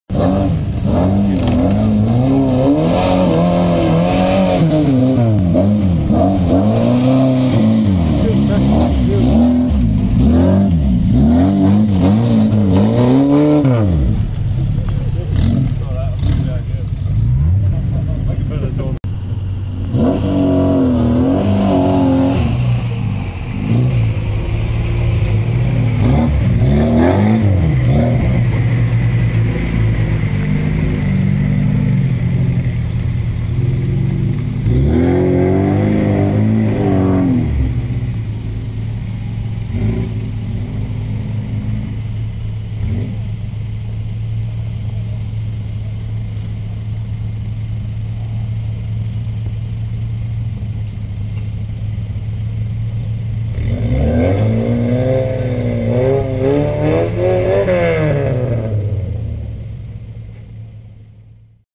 Lada Niva 4X4 Modified Competing 2018 UK Lake District
In an event dominated by Suzuki Jimnys and large all terrain vehicles a Lada Niva turns up to take on the competition.